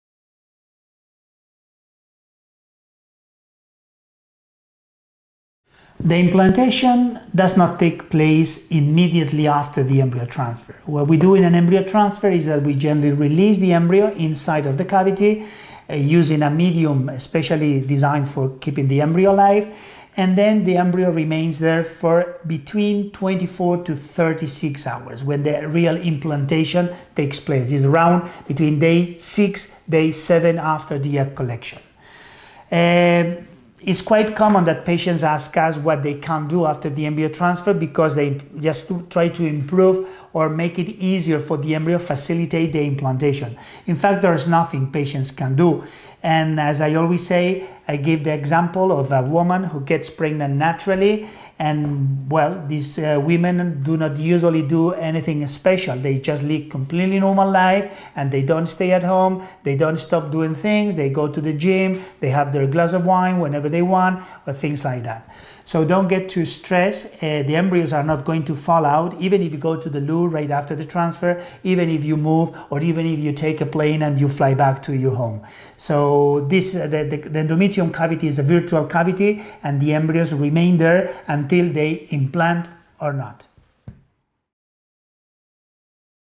How long does it take for an embryo to implant after transfer?
Dr. Raúl Olivares, Medical Director & Owner of Barcelona IVF, Spain.